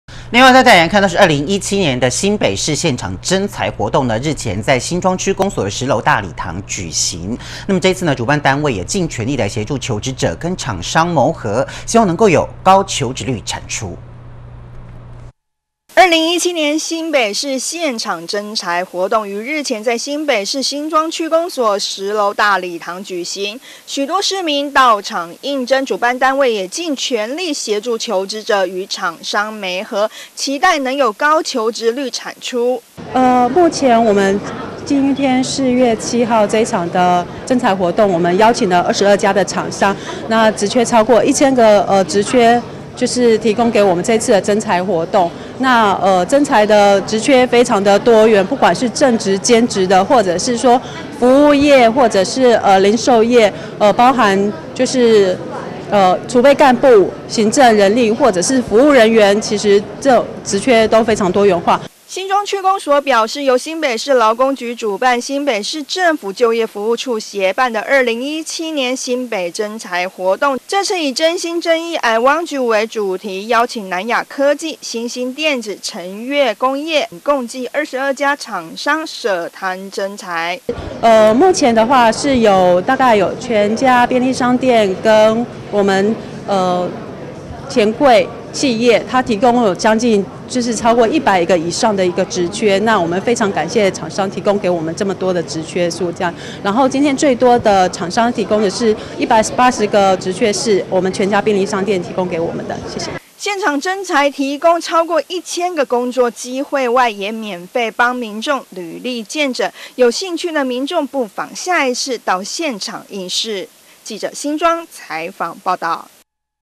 10604142017新北徵才 新莊公所開跑
2017年度薪北市現場徵才活動，於日前在新北市新莊區公所10樓大禮堂舉行。許多市民到場應徵，主辦單位也盡全力協助求職者與廠商謀合，期待能有高求職率產出。